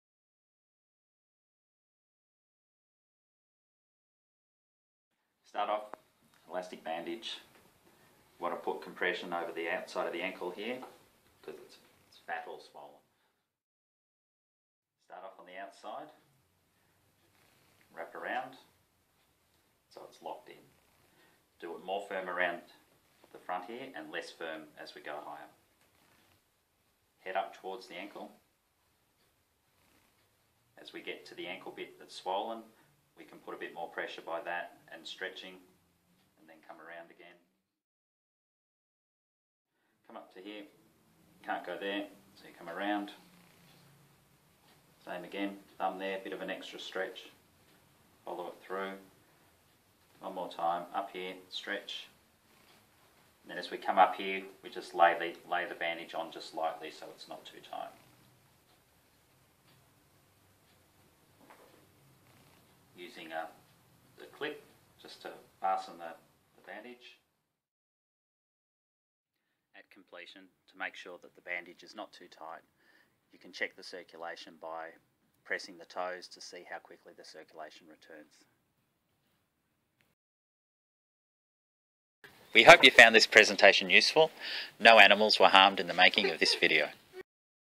How to apply a compression bandage to a sprained ankle
A short educational video on how to apply a compression bandage to an ankle following a sprain.

For four steps on how to manage an ankle sprain visit this

Fountain Gate Physiotherapy
427 Princes Hwy
Narre Warren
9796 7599